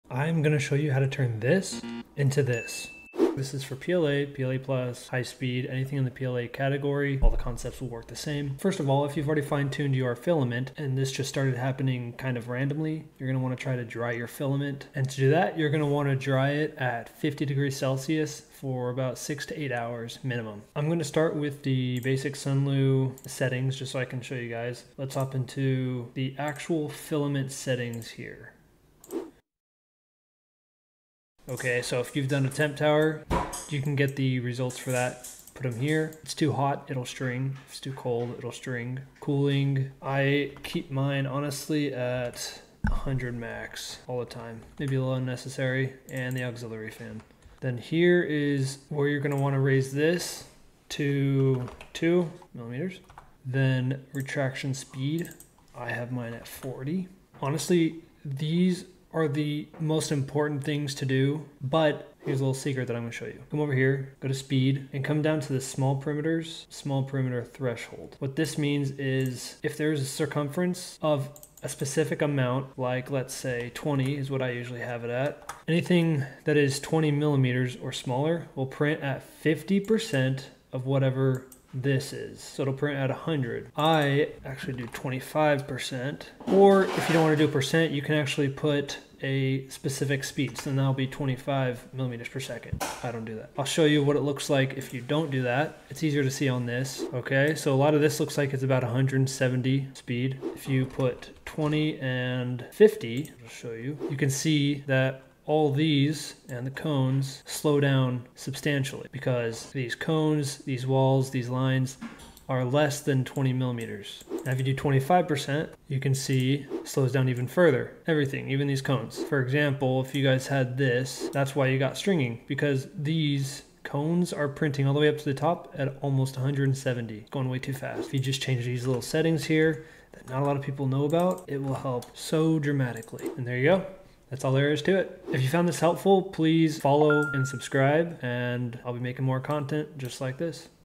Fix Stringing NOW
Just dropped a quick video breaking down my secret sauce for fixing PLA stringing problems 👀 — it’s all about tweaking ________ and _____________ (check out the vid)! These little settings make a big difference when chasing clean prints. Say goodbye to stringy messes! 💪🧵✨